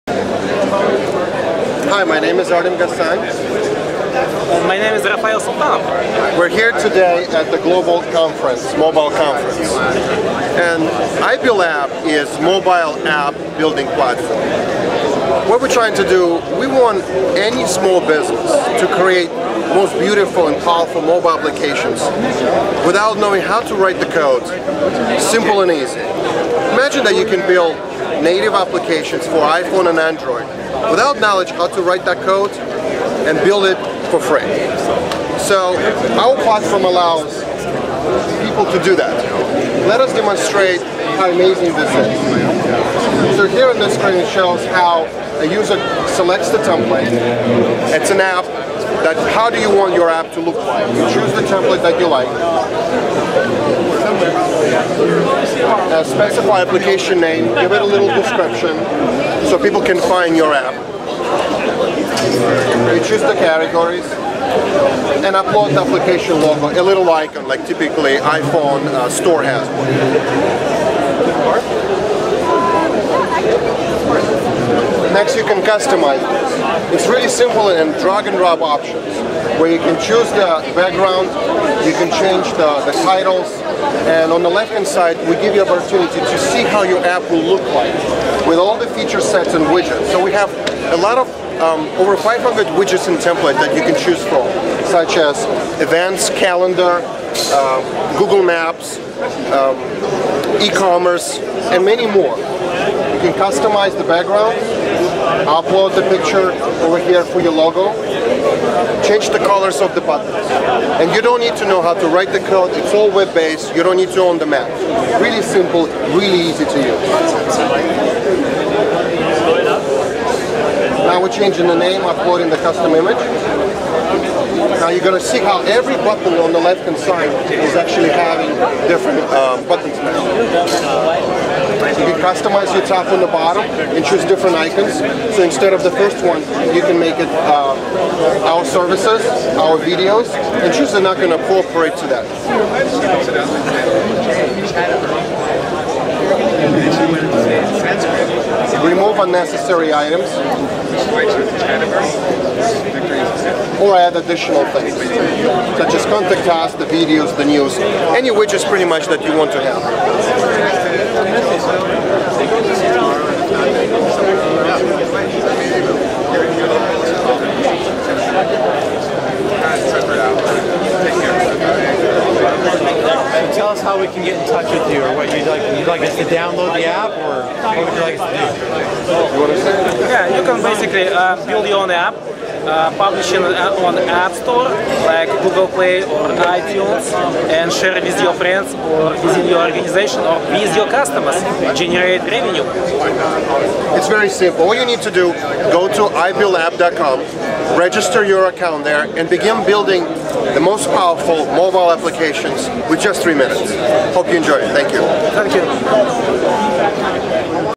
iBuildApp Demo - GMIC-SV
iBuildApp product demonstration at GMIC-SV October 2012

Global Mobile Internet Conference / Great Wall Club Inc (US)

Video Work by:
Troy McGee
Silicon Valley Video Works